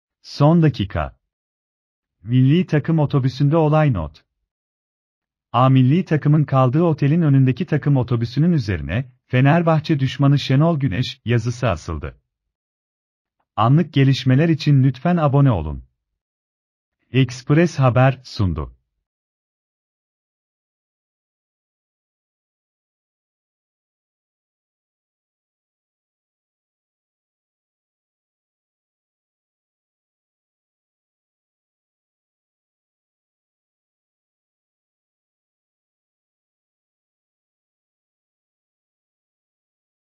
Son dakika... Milli Takım otobüsünde olay not!
A Milli Takım’ın kaldığı otelin önündeki takım otobüsünün üzerine “Fenerbahçe düşmanı Şenol Güneş” yazısı asıldı.